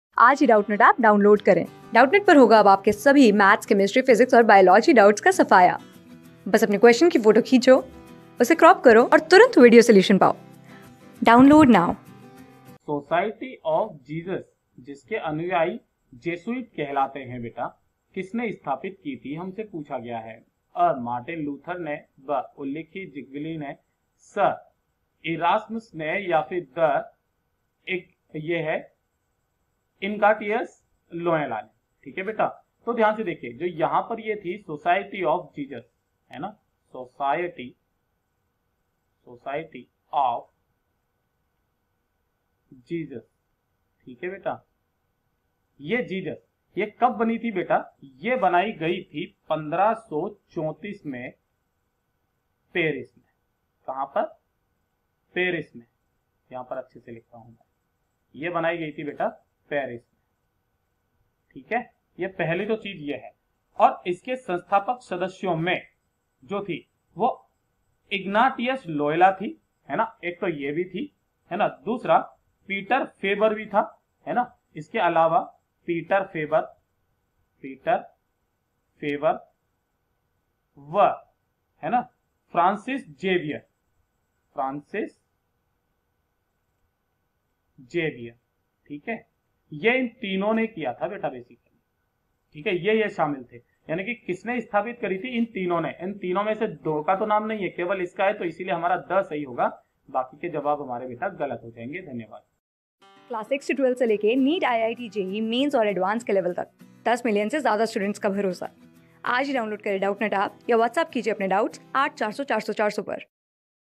सोसाइटी ऑफ जीसस, जिसके अनुयायी जेसूइट कहलाते थे, किसने स्थापित की थी? | CLASS 14 | सामान्य विज्ञ...
सोसाइटी ऑफ जीसस, जिसके अनुयायी जेसूइट कहलाते थे, किसने स्थापित की थी?
Class: 14
Subject: GENERAL KNOWLEDGE
Chapter: सामान्य विज्ञान 2014 (I)
Board:GOVT

👉 Have Any Query? Ask Us.